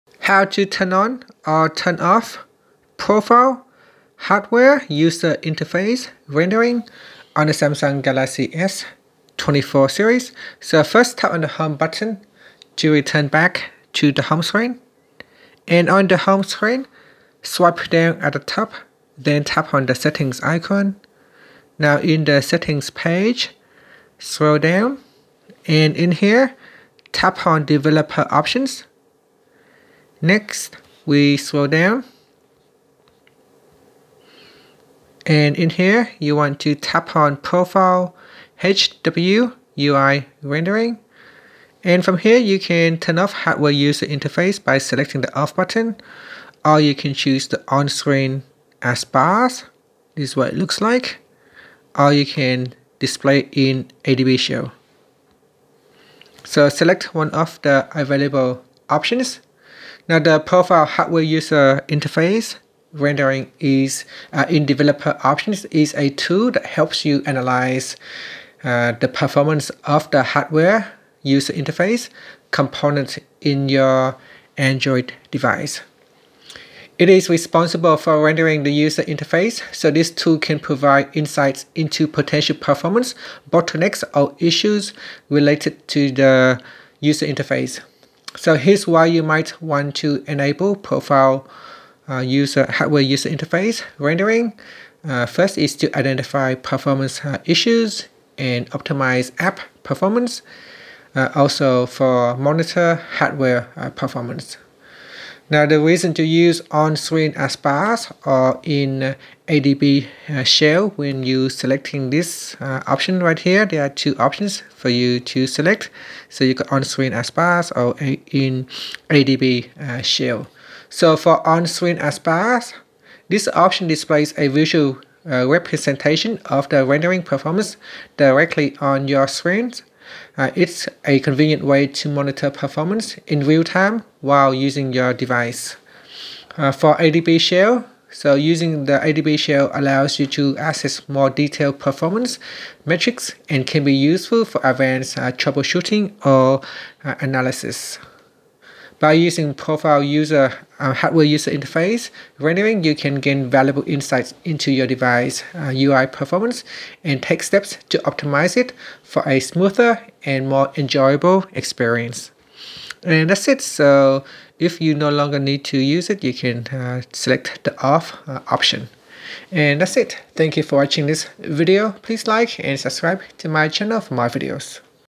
📱Galaxy S24/S24+/Ultra: How to Turn On/Off Profile HWUI Rendering
In this video, I will show you how to turn On/Off Profile HWUI Rendering on the Galaxy S24/S24+/S24 Ultra. By adjusting the HWUI settings, you can monitor GPU performance and ensure smoother app transitions. Perfect for those who love tweaking their phone’s performance for a great experience!